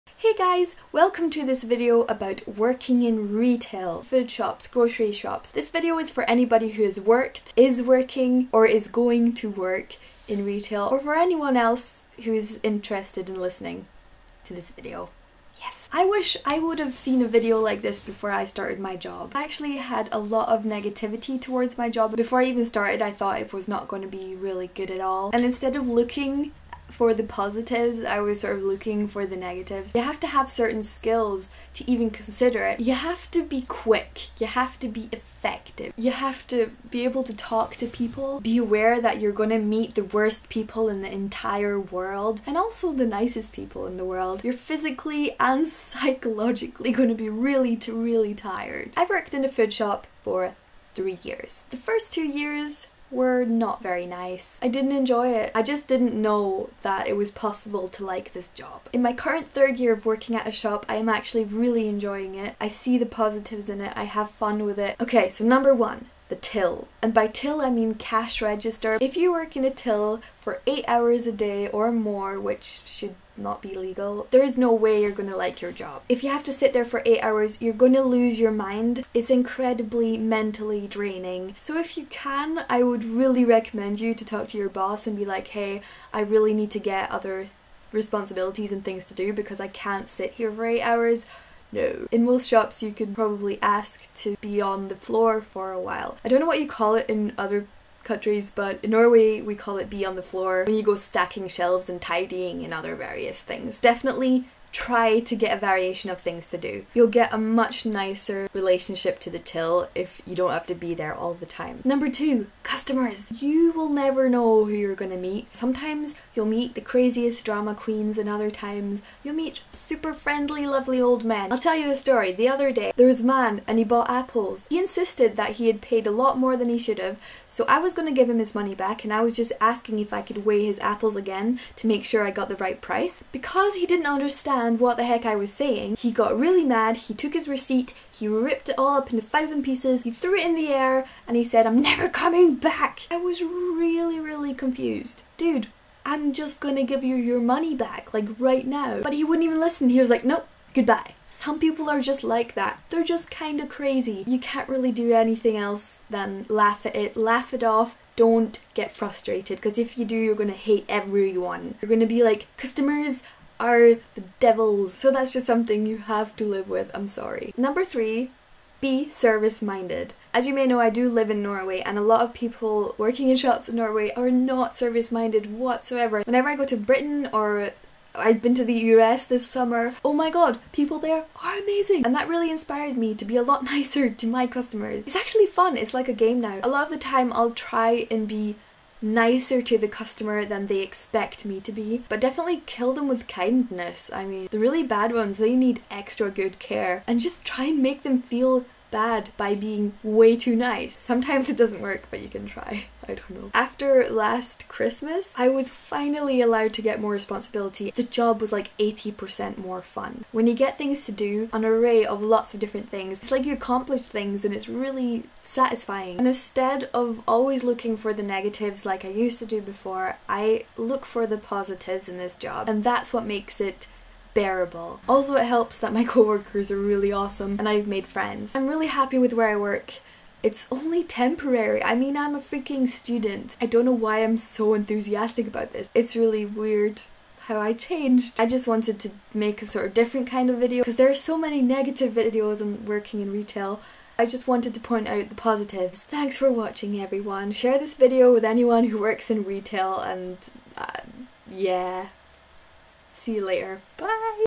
Tips on Working in Retail
Finally a sort of positive video about working in retail. Don't get me wrong, I have had the worst days of my life at this job, but the last year it has been really nice. There is hope... I think!